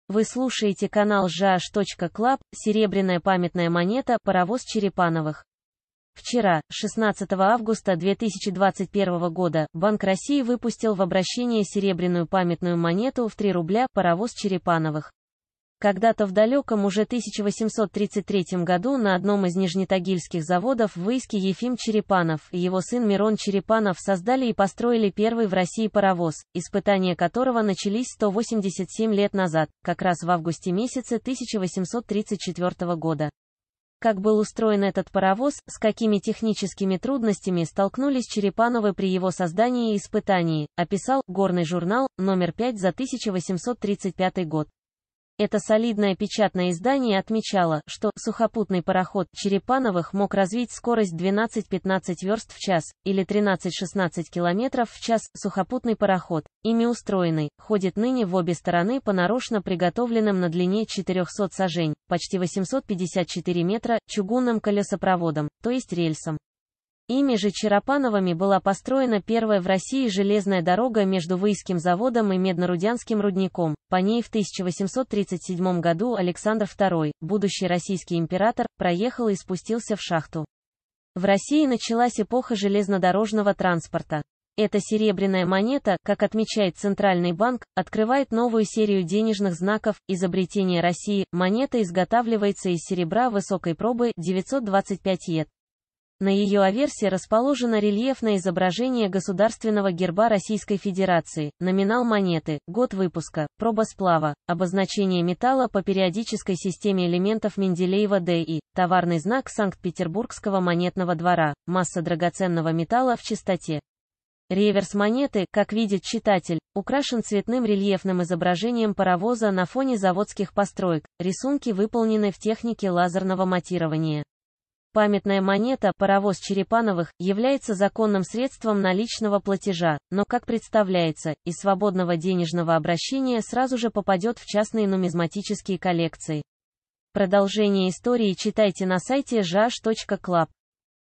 Серебряная памятная монета "Паровоз Черепановых"
Когда-то в далёком уже 1833 году на одном из нижнетагильских заводов в Выйске Ефим Черепанов и его сын Мирон Черепанов создали и построили первый в России паровоз, испытания которого начались 187 лет назад, как раз в августе месяце 1834 года.